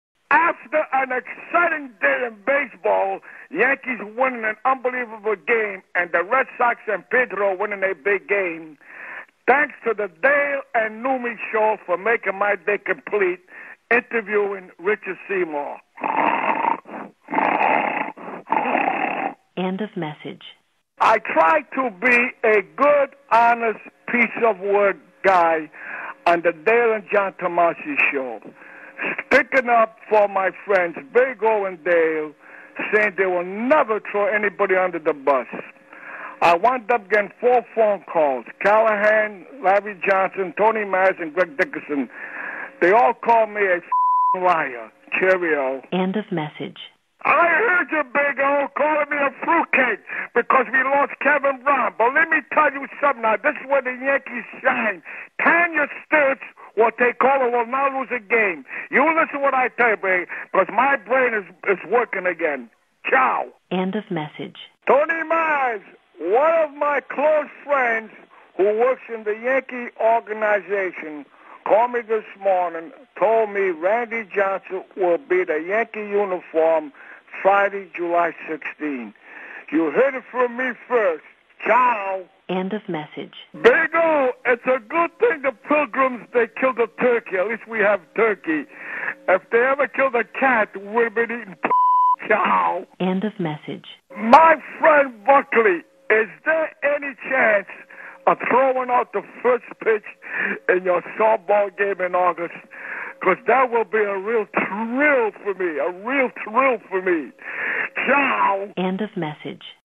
Rest in peace, Frank from Gloucester
Here are some of his best whines.